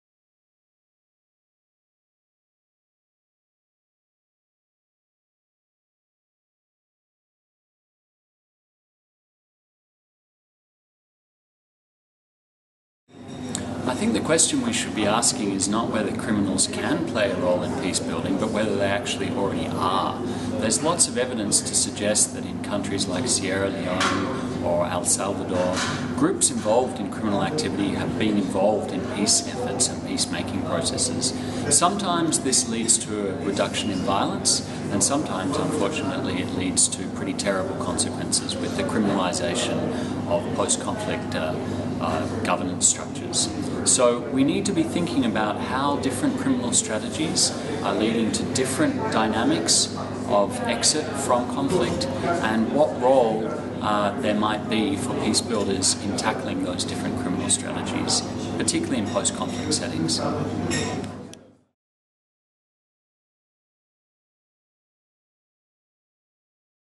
Conflict Ideas Forum - James Cockayne
James Cockayne, Former Co-Director, Center on Global Counterterrorism
Cooperation, New York, answers "Can criminals play a role in peacebuilding?"